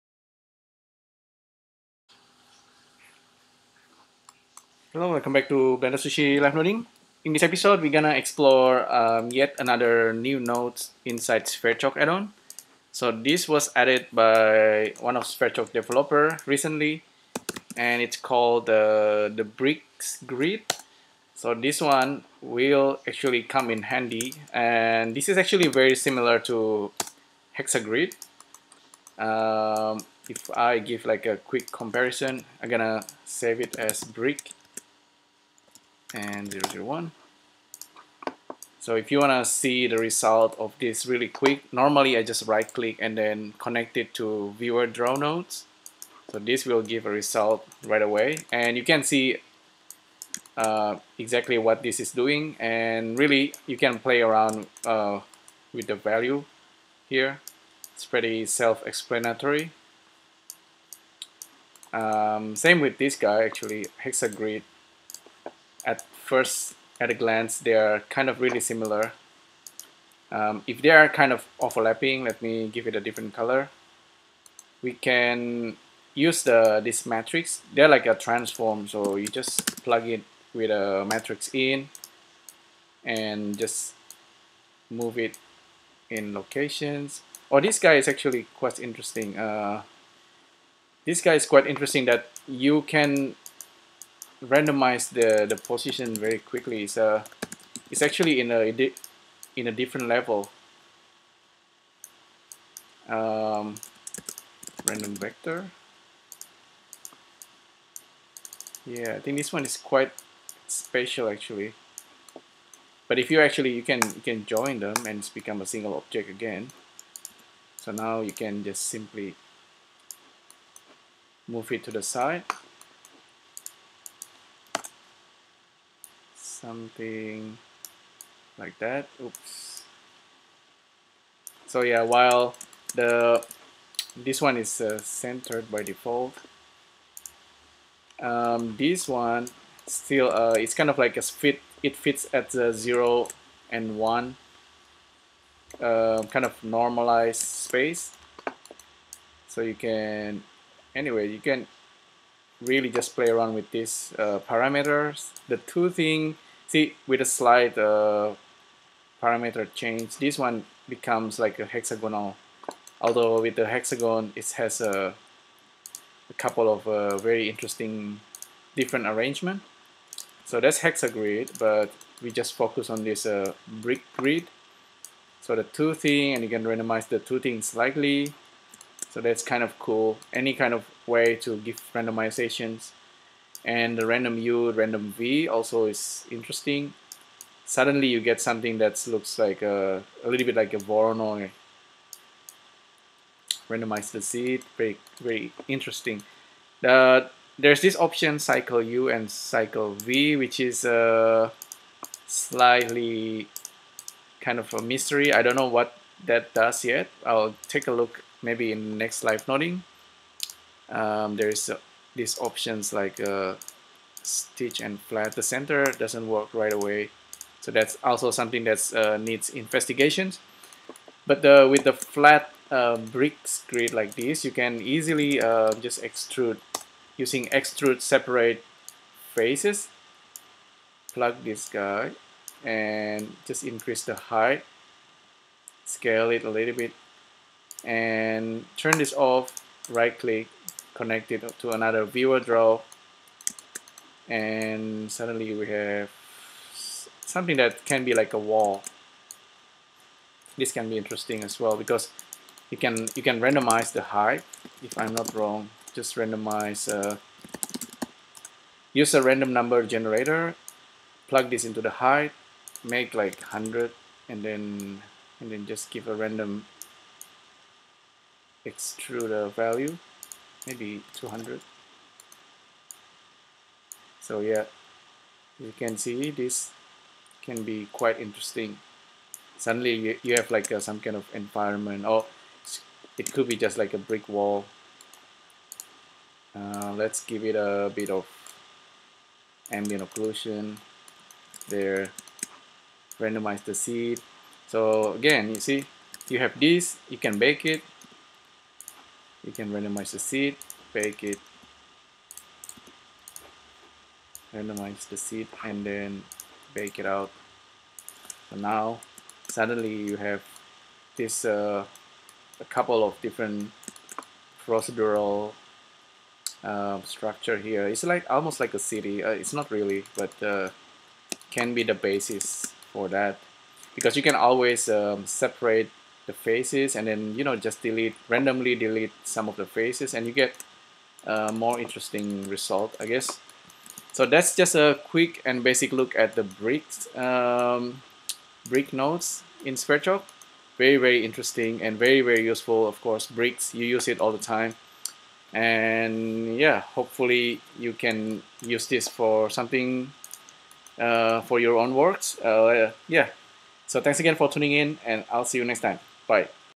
LIVENODING 853 / SV Basic Bricks Node
In this episode, I am taking a quick look at yet another new node for Blender Sverchok Add-On: Bricks Grid node.

The node is fairly intuitive to use. There are still a few things to investigate to take full advantage of the node. But at the very basic level, you could actually use this to built realistic brick walls structure.

I encourage you to test this node yourself, check the documentation and the development thread, you may discover more interesting features: